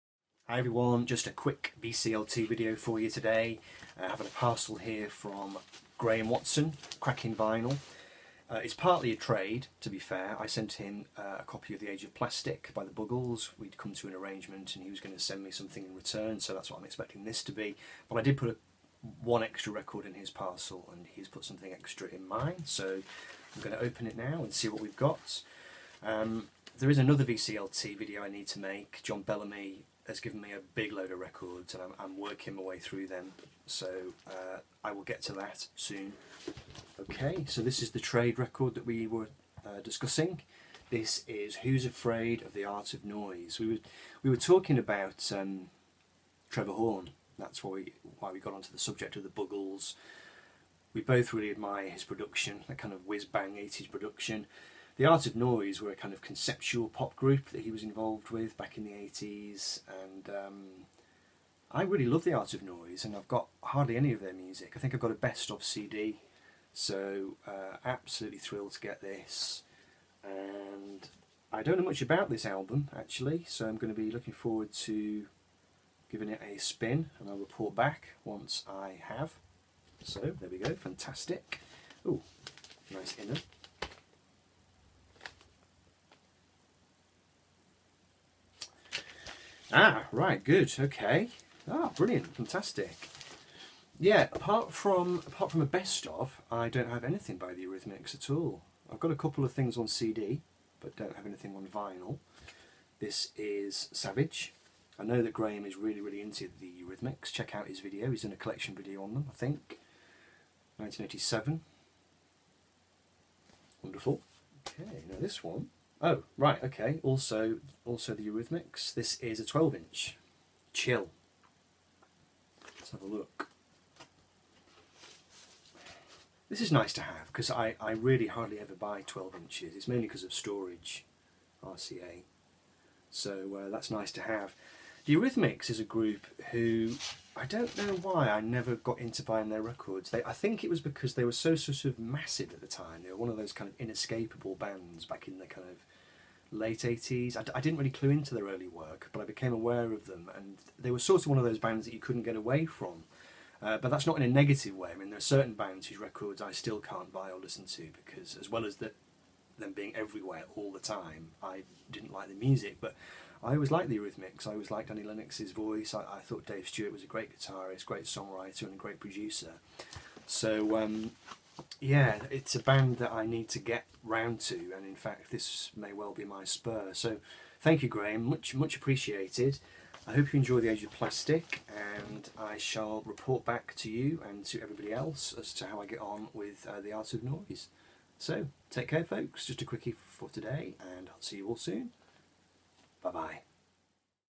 Artful VCLT From Crackin' Vinyl
Showing 3 records sent to me recently by the inimitable and inspirational Graeme Watson. check him out here: